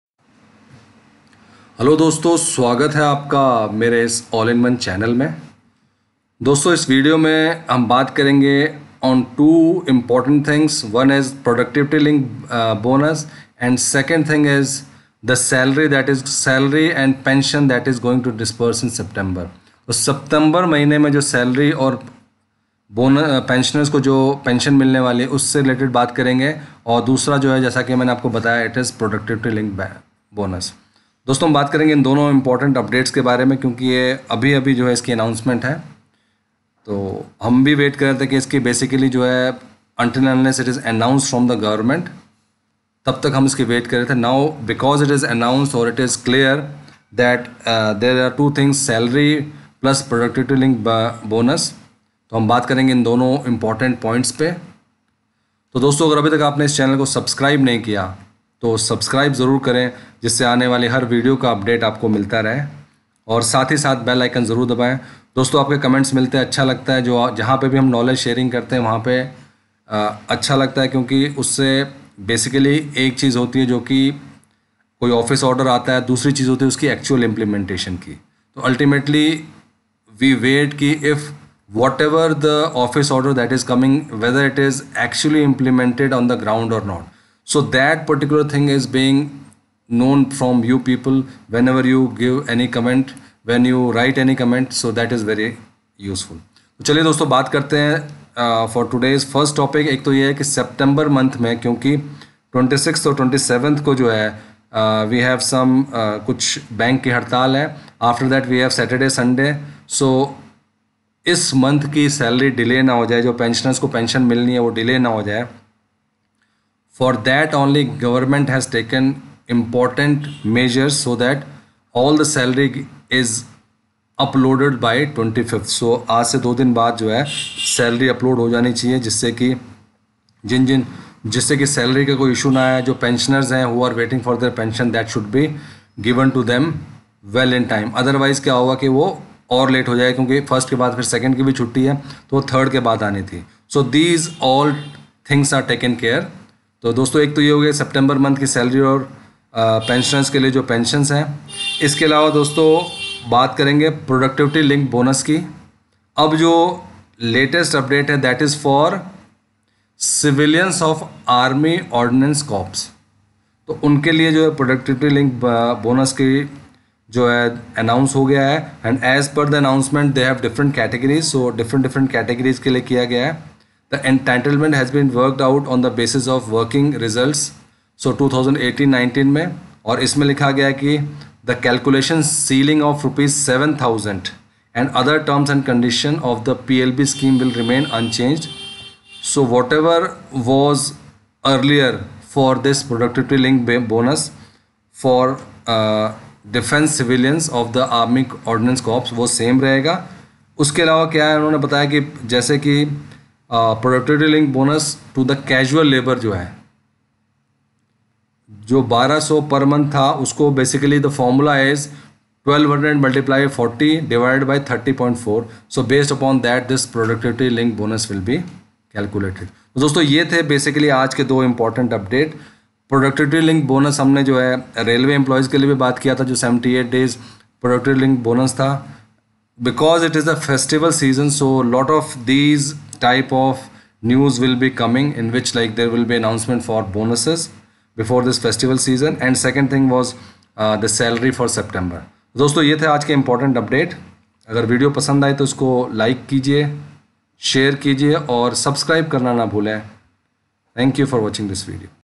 Pensioners Good News!! सभी केंद्रीय कर्मचारीयों और पेंशनर्स के लिए आज की बड़ी खबर
In this video for pensioners and govt employee for salary and pensions for central govt employees and pensioners.